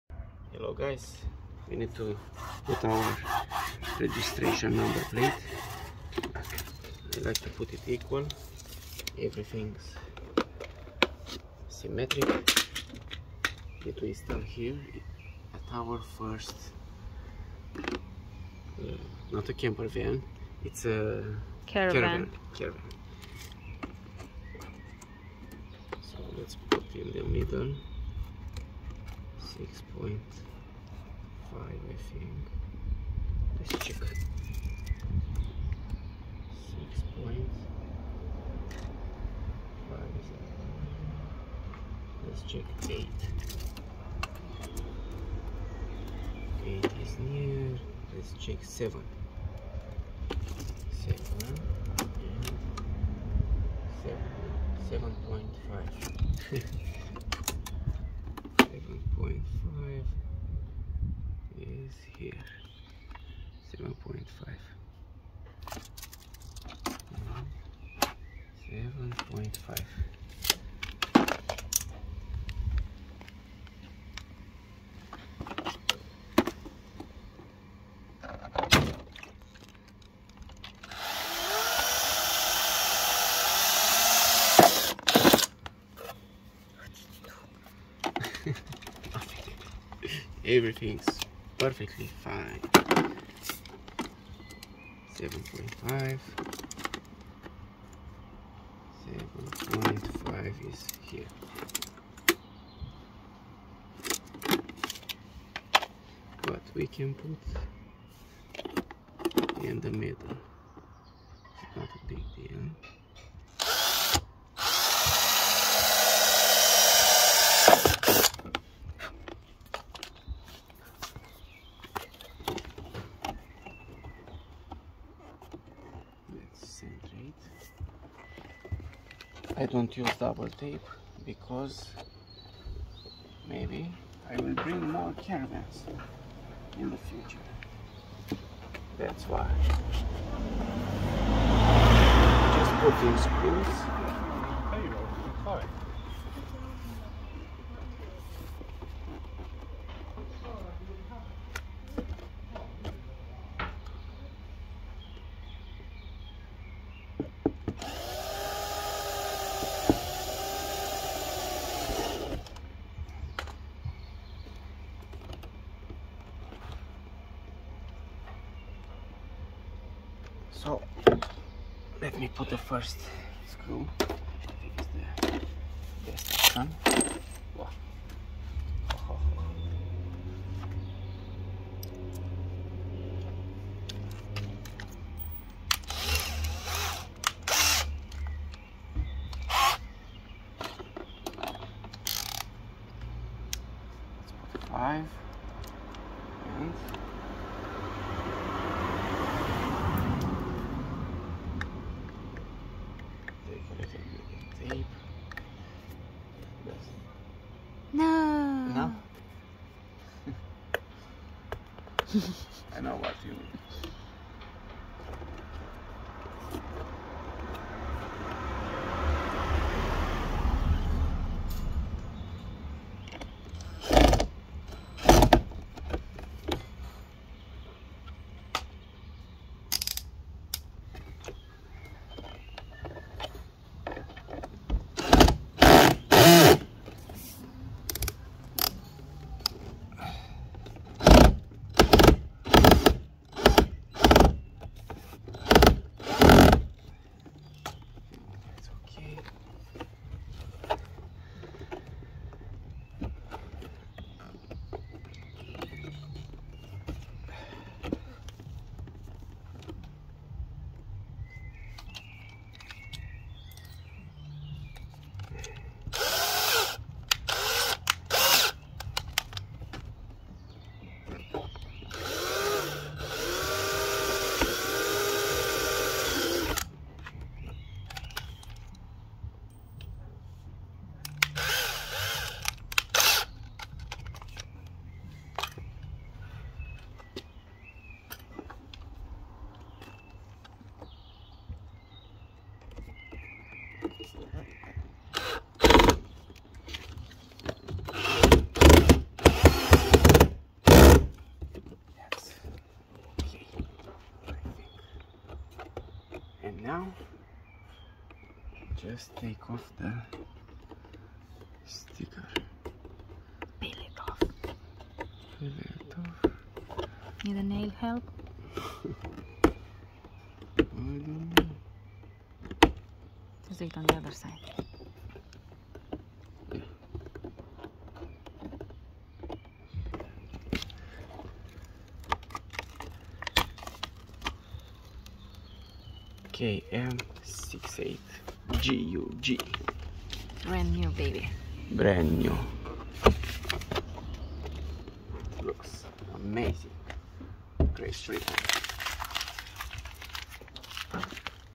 Cea mai rapida si simetrica metoda sa instalezi nr inmatriculare la Rulota
Exact si Corect placute inmatriculare